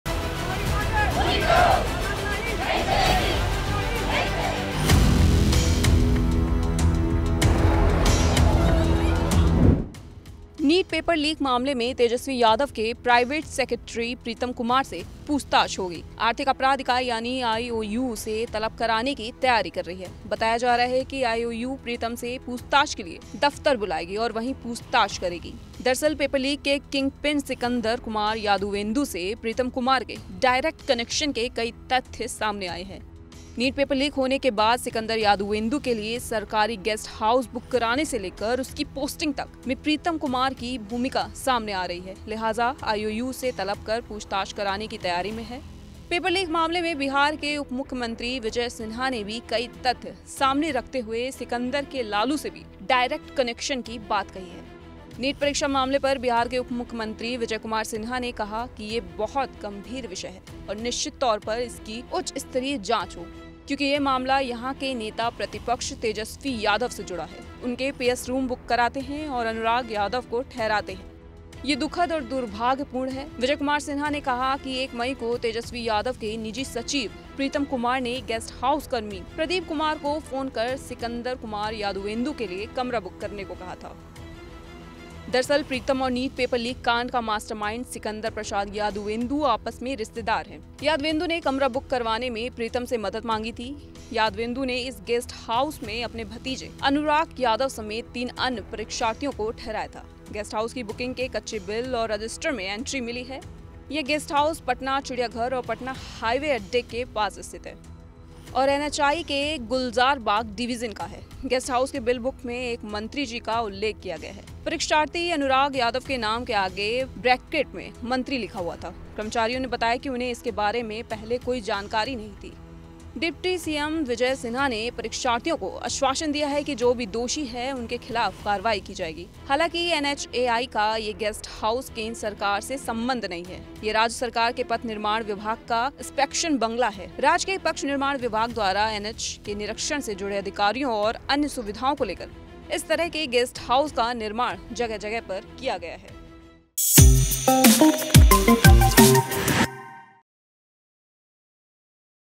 NEET SCAM EXPOSED : फंस गए तेजस्वी के PS प्रीतम कुमार, EOU कर रही है पूछताछ की तैयारी! | ABP LIVE
News: नीट पेपर लीक मामले में तेजस्वी यादव के प्राइवेट सेक्रेटरी प्रीतम कुमार से पूछताछ होगी. आर्थिक अपराध इकाई यानी EOU उसे तलब करने की तैयारी कर रही है. बताया जा रहा है कि EOU प्रीतम से पूछताछ के लिए इओयू दफ्तर बुलाएगी और वहीं पूछताछ करेगी